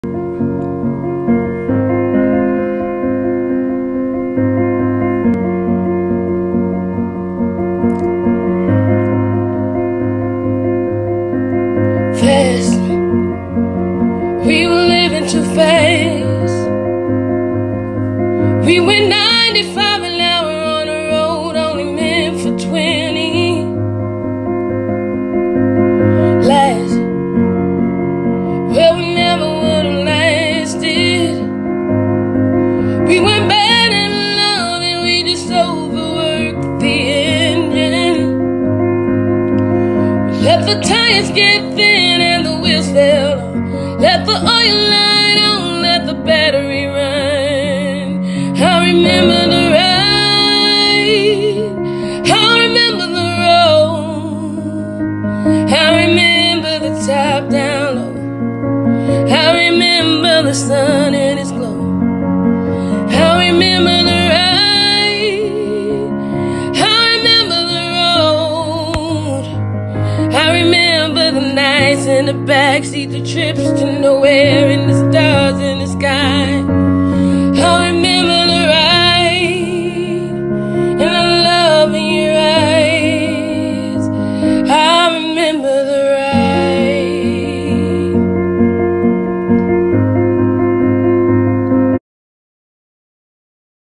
This song is for anyone dealing with the aftermath of a crash and burn relationship .
Going to start posting voice memos up here I have about 1,899 of them at this point . I'll call these the Voice Memo Chronicles

Fast ,
We were living too fast
We went 95 an hour on road meant for 20

Last ,
Well we never would’ve lasted
We weren’t bad at loving we just  over worked the engine .

pre chorus

We let the tires get thin and the wheels fell off .
We left the oil light on . We let the battery run .

Chorus
I’ll remember the ride I’ll remember the road , I’ll remember the top down low . I’ll remember the sun and it’s glow .ill remember the ride I the road I’ll remember the ride , I’ll remember the road , I’ll remember nights in the back seat , trips to nowhere and the stars in the sky .
I’ll remember the ride And the love in your eyes . I’ll remember the ride .